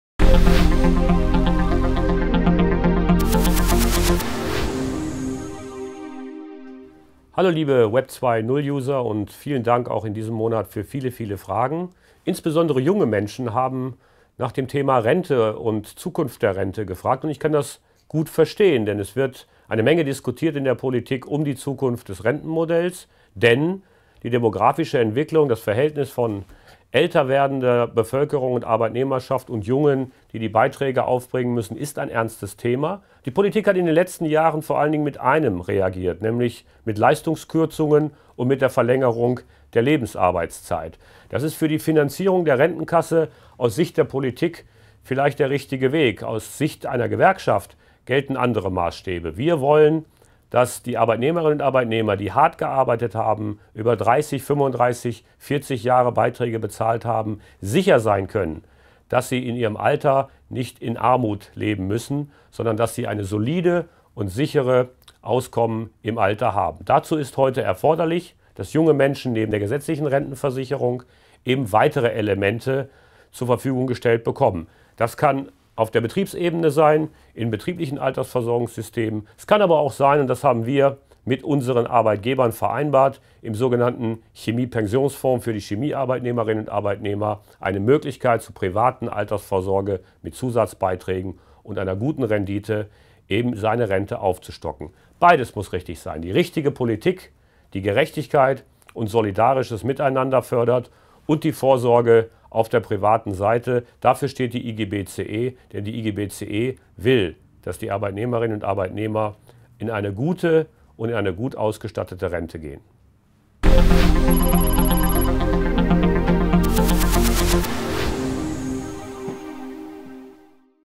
Eine Frage, Herr Vassiliadis! Thema: Rente
Saskia Meyer (23): "Kriege ich später überhaupt noch meine Rente?"